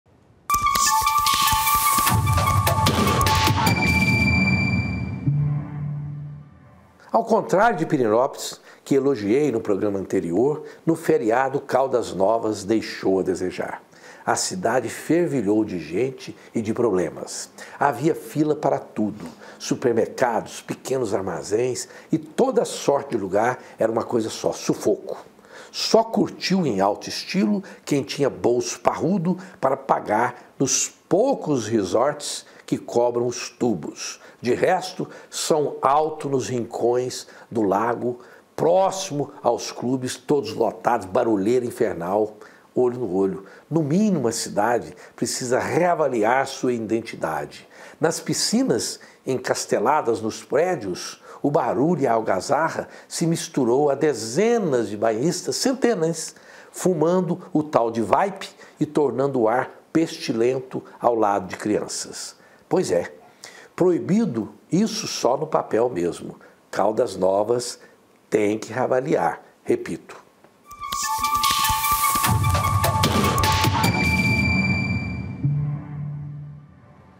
OLHO NO OLHO: CALDAS NOVAS PRECISA REAVALIAR PERFIL TURÍSTICO
OLHO NO OLHO. O JORNALISTA ROSENWAL FERREIRA COMENTA COMO F0I O FERIADO PROLONGADO NAS PRINCIPAIS CIDADES TURÍSTICAS GOIANAS. ENQUANTO PIRENÓPOLIS FOI ELOGIADA, CALDAS NOVAS RECEBEU MUITAS CRÍTICAS .